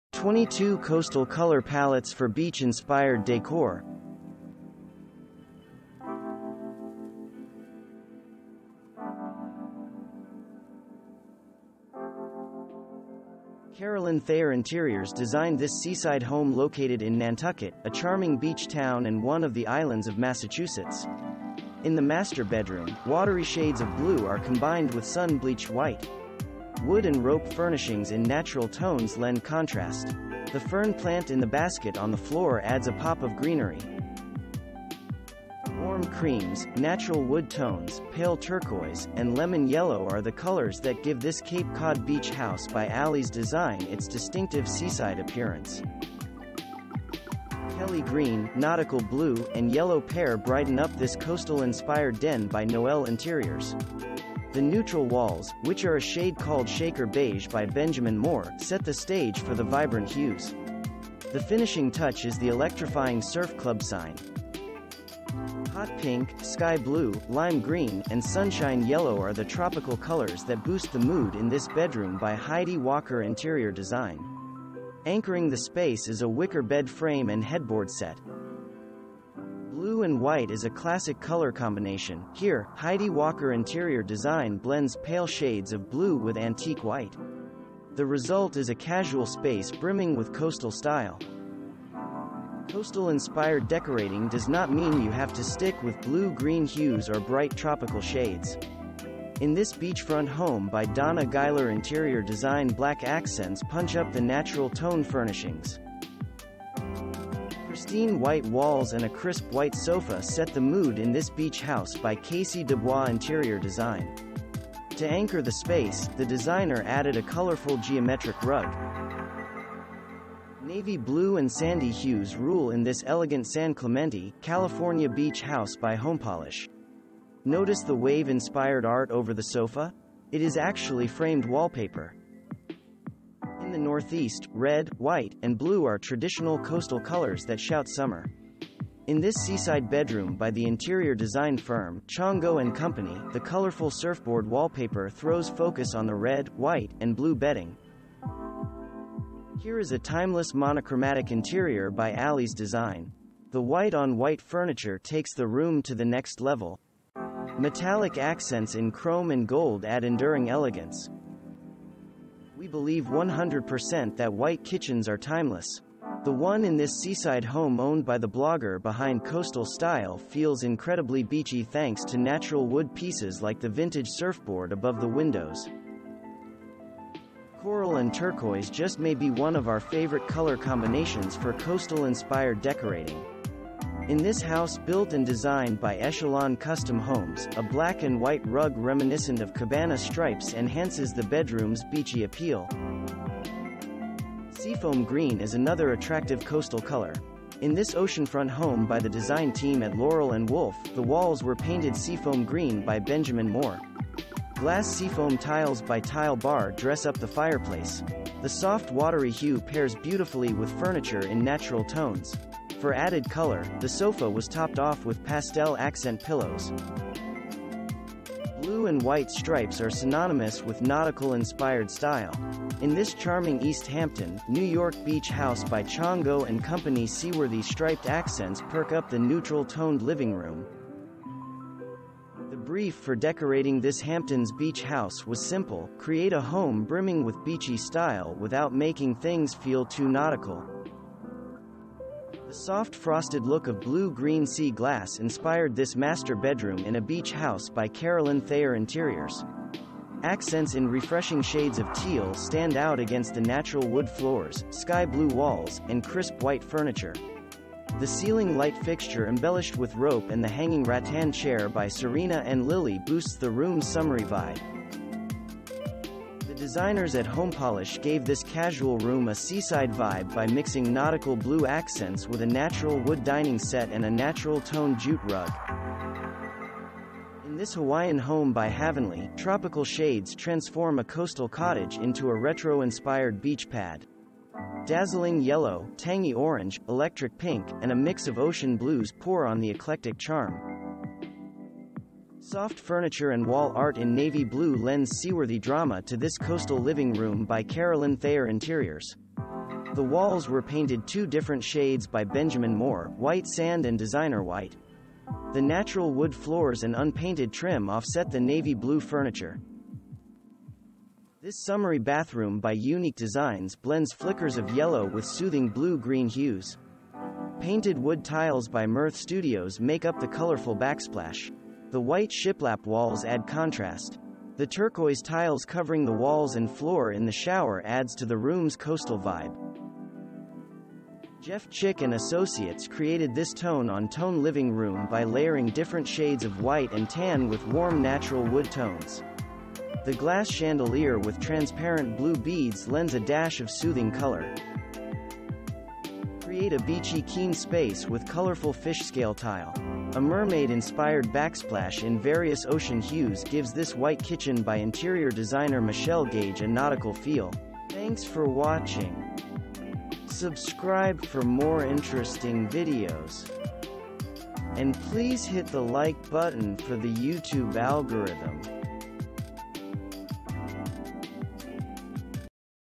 22 Coastal Color Palettes For Beach Inspired Decor 🧧
To help you capture the spirit of summer in your abode year round, here are the breeziest color combos for coastal-inspired decorating.
0:00 - Intro
0:00:12 -   Beachy Blue Colors
0:00:21 -   Cape Cod Beach House
0:00:30 -   Nautical Blue, Kelly Green, and Yellow Pear
0:00:39 -   Perk Up a Room With Tropical Colors
0:00:48 -   Blue and White Color Scheme
0:00:57 -   Punch Up a Neutral Space With Black Accents
0:01:06 -   Crisp White Living Room
0:01:15 -   San Clemente Beach House
0:01:24 -   Red, White and Blue
0:01:33 -   Monochromatic Beach House
0:01:42 -   Combine White With Natural Wood Tones
0:01:51 -   Coral and Turquoise
0:02:00 -   Seafoam Green
0:02:09 -   Seaworthy Blue and White Stripes
0:02:18 -   Bright White Indigo Blue
0:02:27 -   Sea Glass Blue
0:02:36 -   Natural Tones and Nautical Blue
0:02:45 -   Retro Inspired Tropical Beach House
0:02:54 -   Navy Blue for Seaworthy Drama
0:03:03 -   Add a Splash of Tangy Yellow
0:03:12 -   Tone on Tone Living Room
0:03:21 -   Fish Scale Tile in Ocean Hues